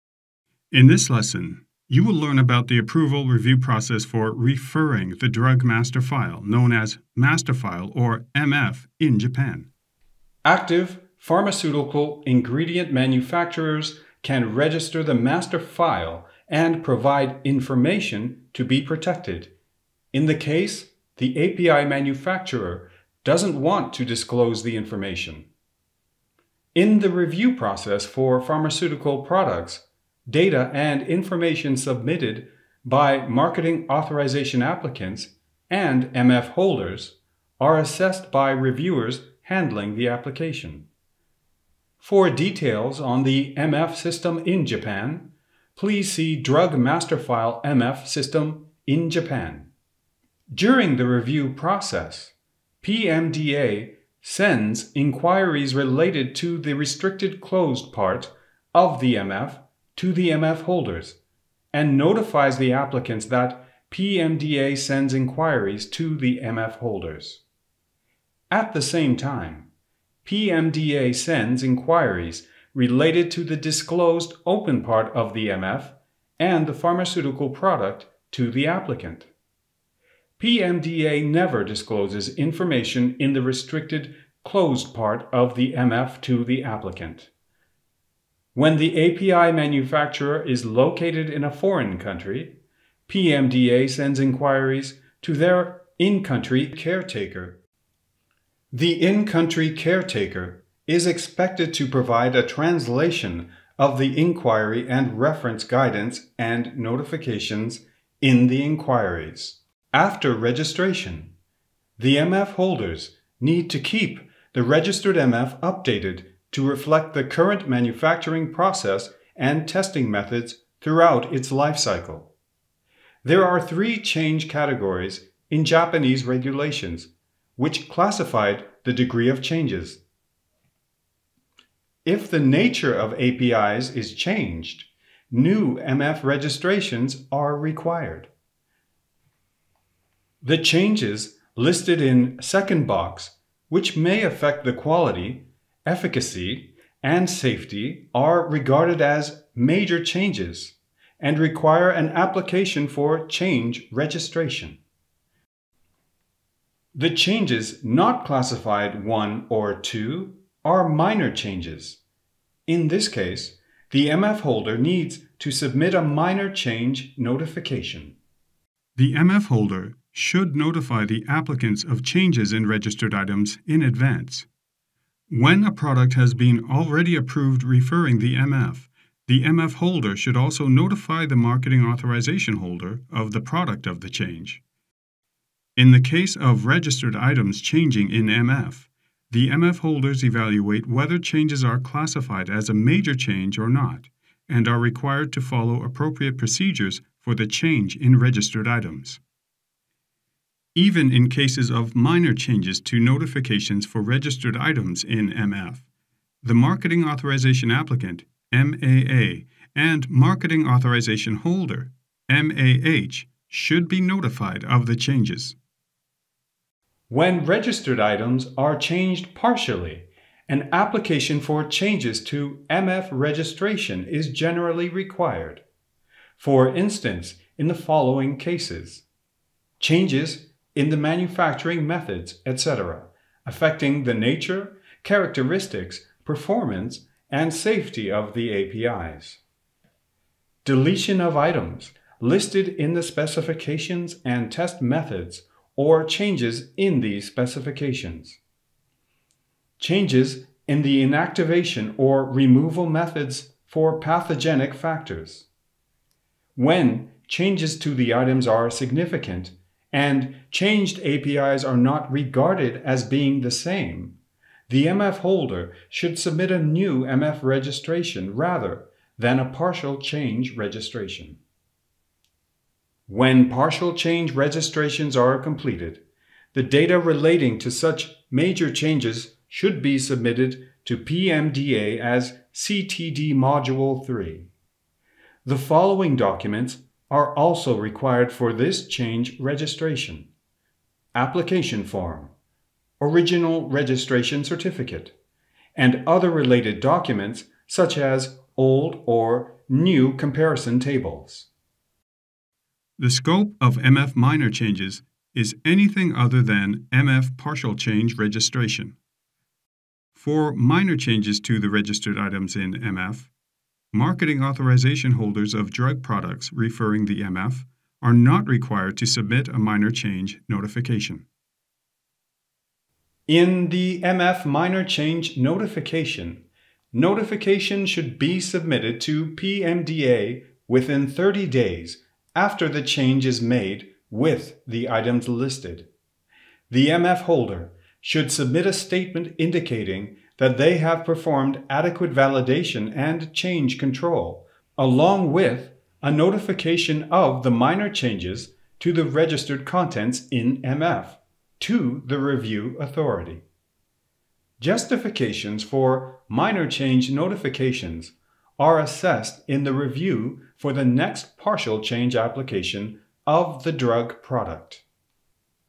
(Review) Approval review referring Drug Master File - PMDA-ATC Learning Videos
This video introduces the process of approval review and change registration for the pharmaceutical products referring Master File (MF).
For details on the MF system, please see “Drug Master File System in Japan.”